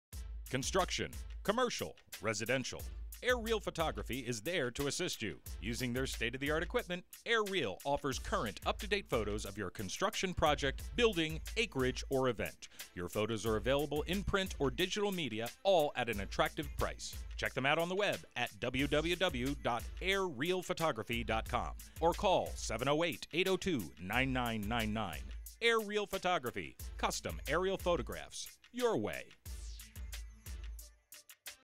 Air Real Photography TV commercial
Construction.  Commercial.  Residential.  Events.
Air Real Photography is there to assist you.
Air Real is a custom aerial photography service.
Using their state of the art equipment, Air Real offers current up to date photos of your construction project, building, acreage, or event.  Your photos are available in print or digital media, all at an attractive price.
Air Real Photography.  Custom aerial photographs, - your way!